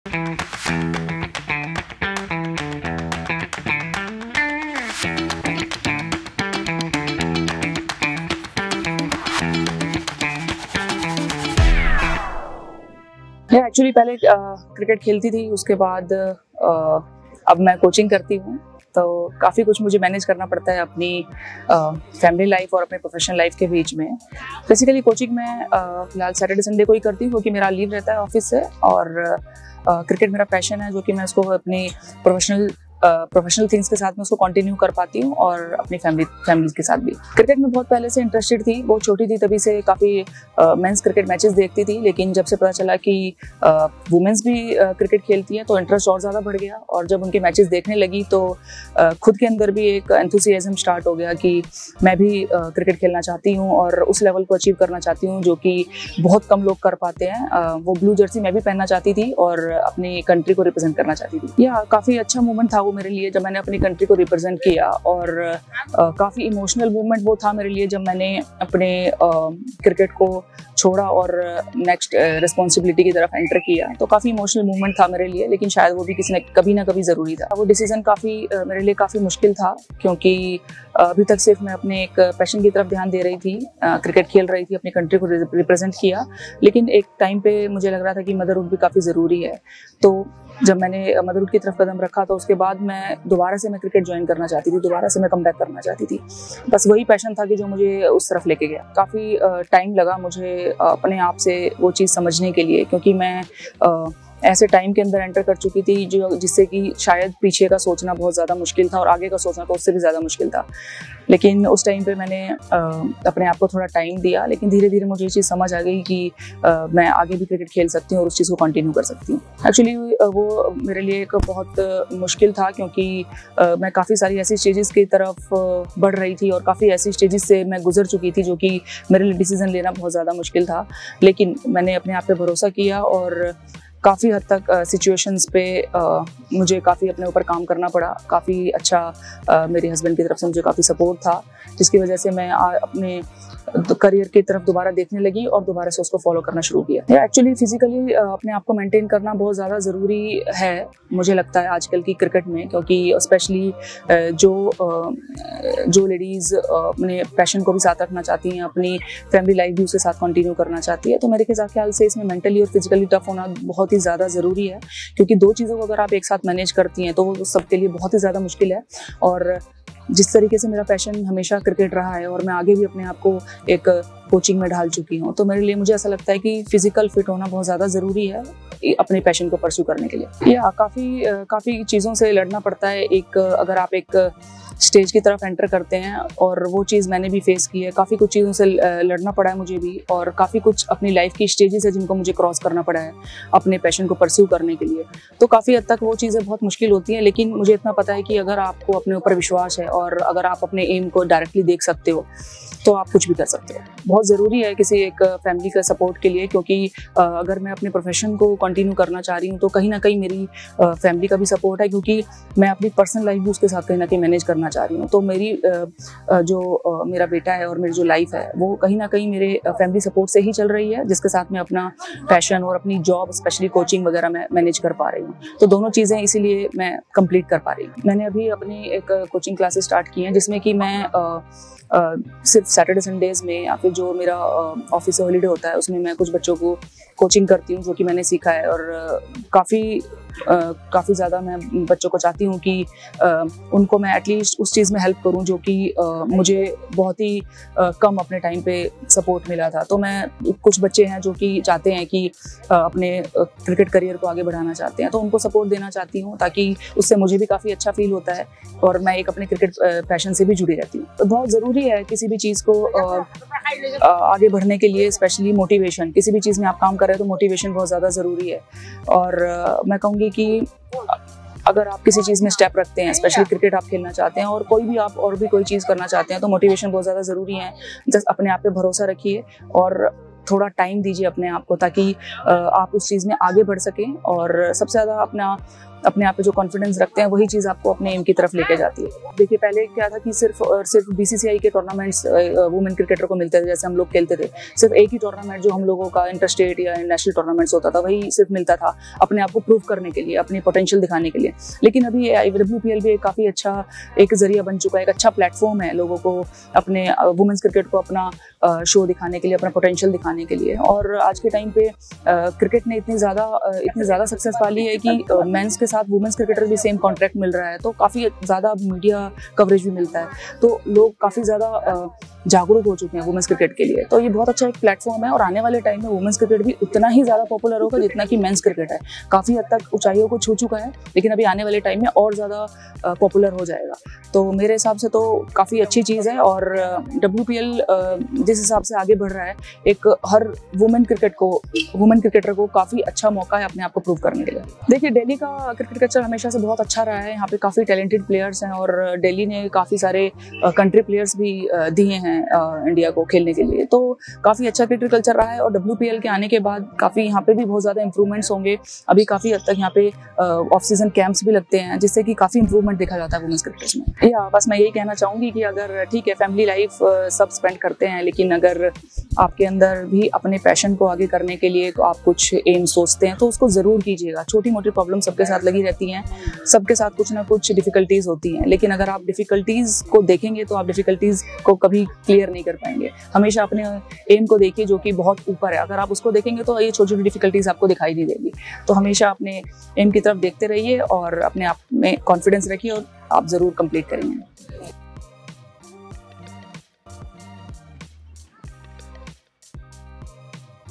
Supermom Neha Tanwar | Interview
An India international who had to leave cricket due to pregnancy but returned to represent India A just three years after becoming a mother  🇮🇳 ❤️

Watch our interview with the Supermom Neha Tanwar!